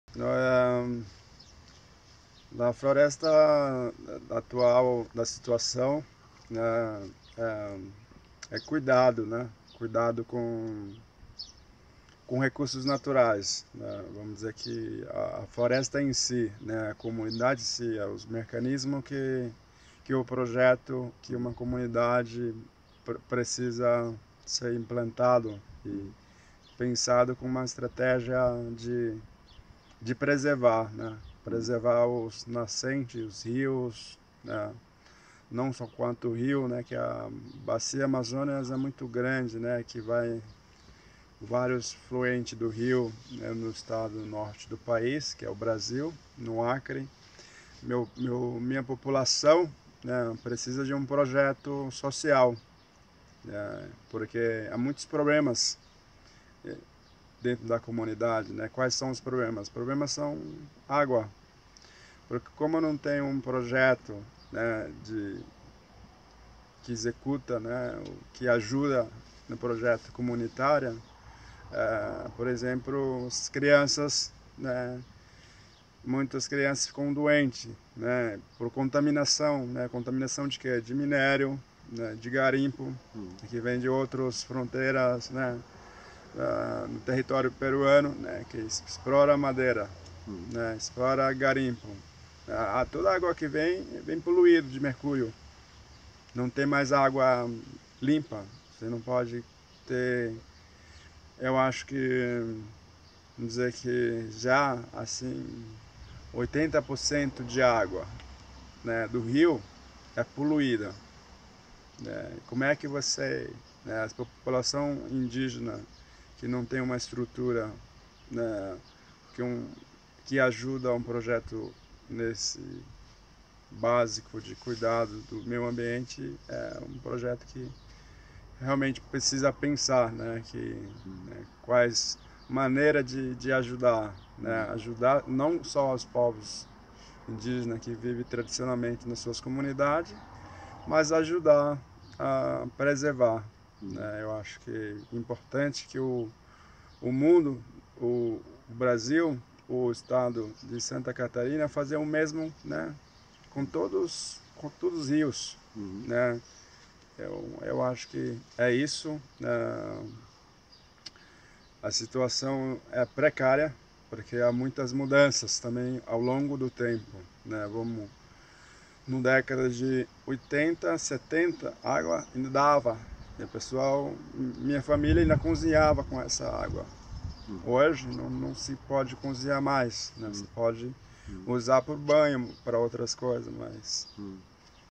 Leopardo Yawa Bane, candidato al parlamento de Brasil 2018 con Rede do Sustentabilidade
Leopardo Yawa Bane, 36 años, hijo del gran jefe Xia de los Huni Kui, tiene una misión: salir elegido diputado federal en las elecciones del próximo 4 de octumbre en Brasil y convertirse así en el primer indígena en sentarse en el parlamento de Brasilia. En juego está la supervivencia de su pueblo, los Hana Kui (“los hombres verdaderos”), de la nación indígena, y, finalmente, la propia “floresta”, la selva amazónica, hogar de su pueblo y penúltima reserva natural del planeta.